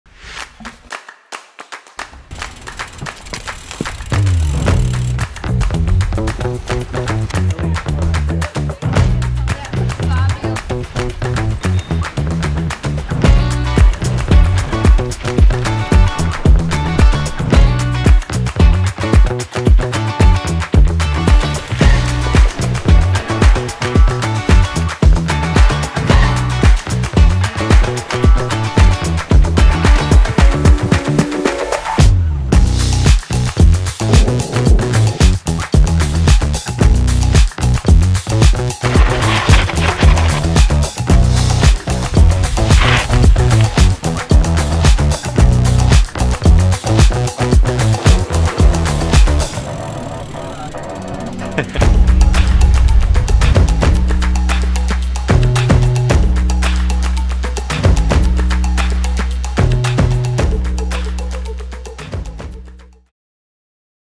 THIS IS GRAVEL — Introducing Cesar Villalba
Big city life and outdoor life sometimes don’t mix. But with a little creativity and motivation you can find that sweet piece of gravel road that takes you away from the hustle and bustle and into the flow mode. Let Cesar Villalba take you for a leopard print pimped ride through some LA gravel paths.

What’s also good about this duality? Training your dismounts, feeling cheeky crossing garden gates, and of course, the best pre and after-ride food stops.
So, grab your helmet and a coffee. Take advantage of your city, get out there, and make your own fun, your own way, always in great style.

Filmed by @ alex_colorito